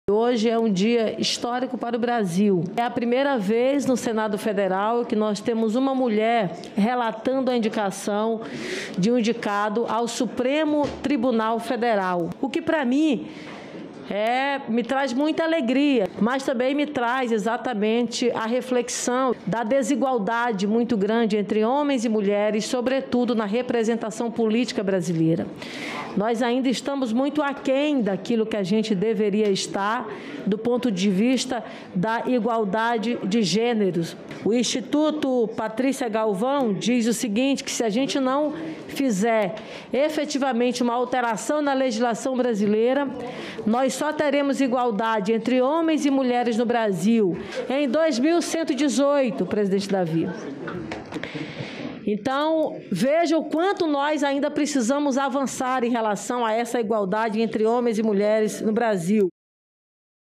Senadora Eliziane Gama é primeira mulher a relatar indicação ao
Durante sabatina do ex-ministro, André Mendonça, a senadora Eliziane Gama (CIDADANIA-MA), primeira mulher a relatar indicação de ministro para o Supremo Tribunal Federal, disse que hoje é uma dia histórico que traz reflexão sobre a igualdade de gênero. Para ela, é preciso avançar nessa questão.